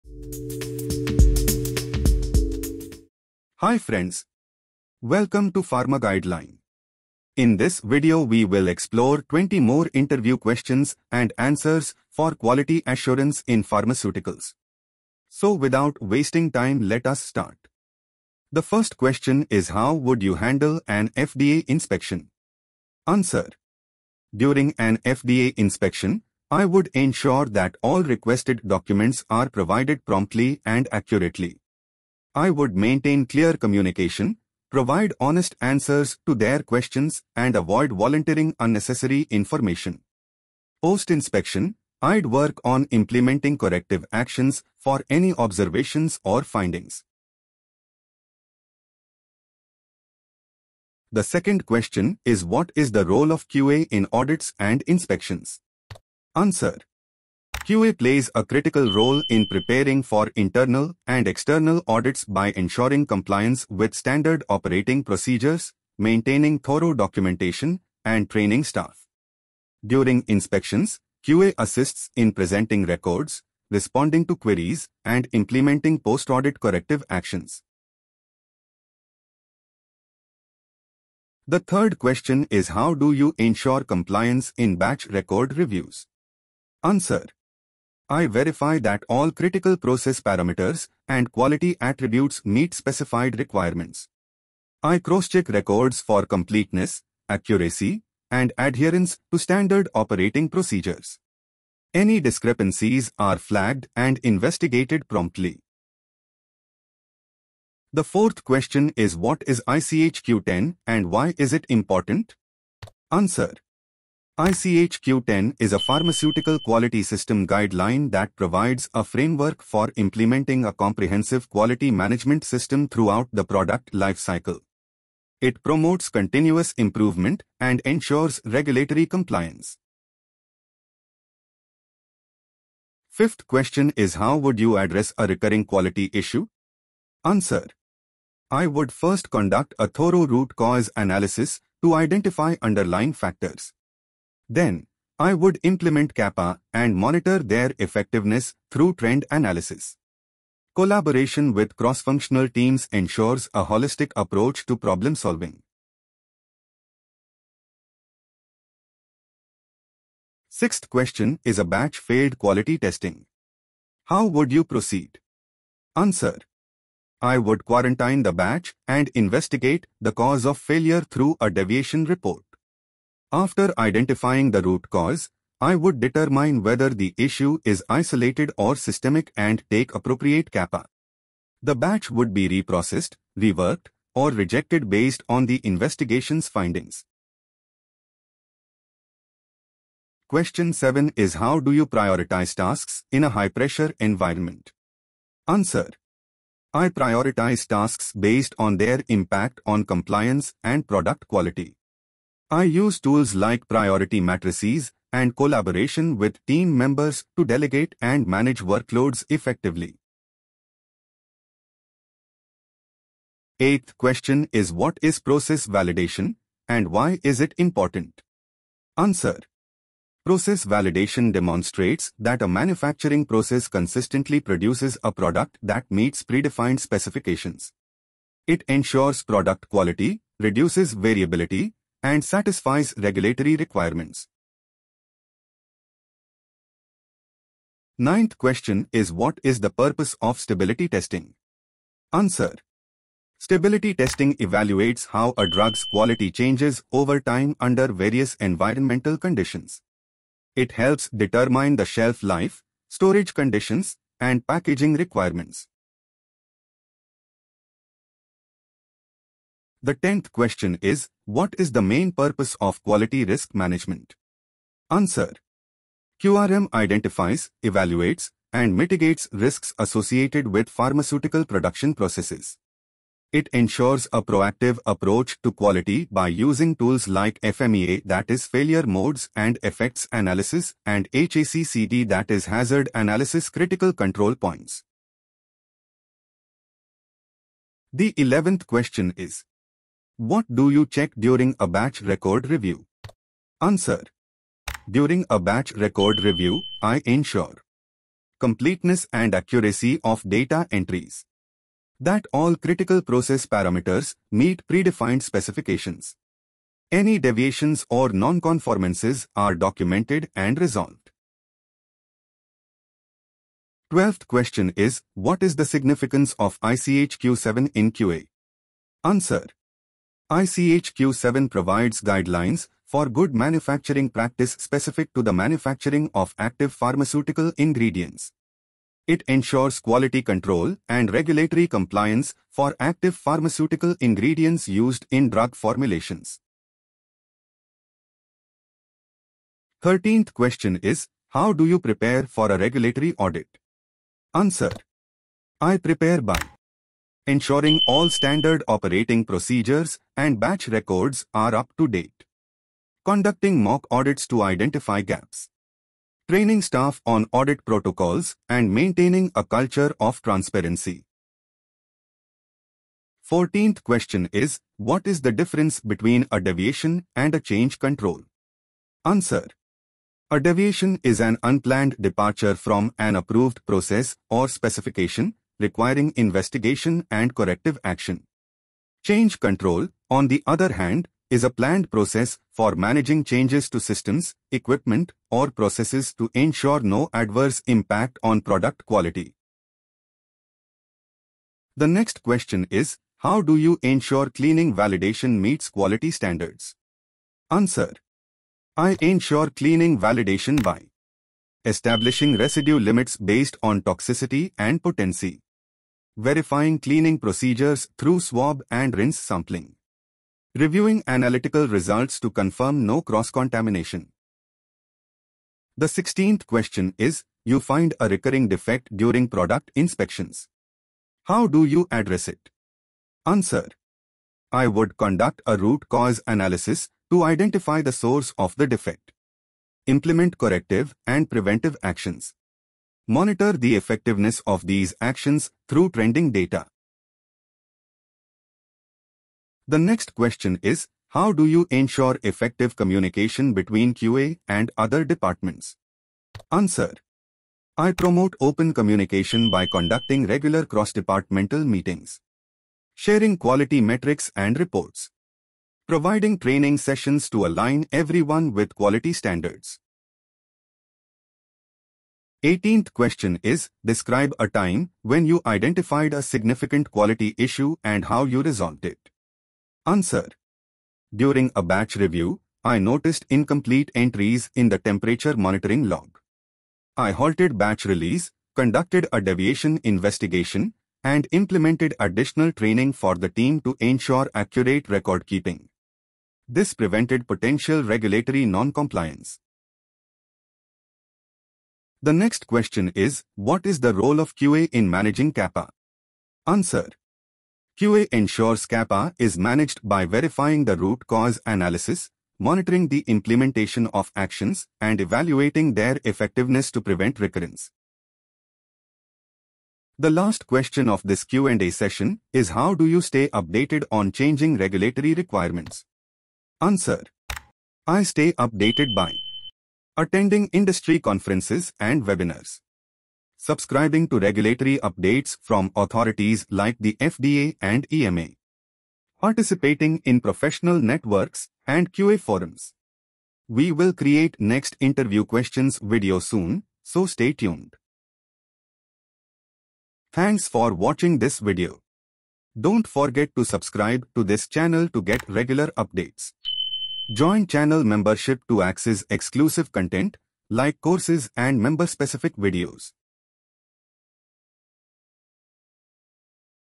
QA Interview Q&A Part 2 | Pharmaceuticals Job Preparation | QA Interview Answers
Are you preparing for a Quality Assurance (QA) job interview in the pharmaceutical industry? In this video, we walk you through the top 20 QA interview questions and answers, explained in a simple and practical way — just like a trainer teaching a student.

Whether you're a fresher or have some experience, this video covers everything from GMP, deviations, CAPA, change control, to validation and documentation.

🎓 What you’ll learn:
Commonly asked QA interview questions
Best way to answer QA questions with confidence
Real-time examples for better understanding
Bonus tips to crack pharmaceutical QA interviews

📚 Perfect for:
Pharmacy graduates, QA professionals, freshers in pharma, and anyone looking to enter the pharma quality domain.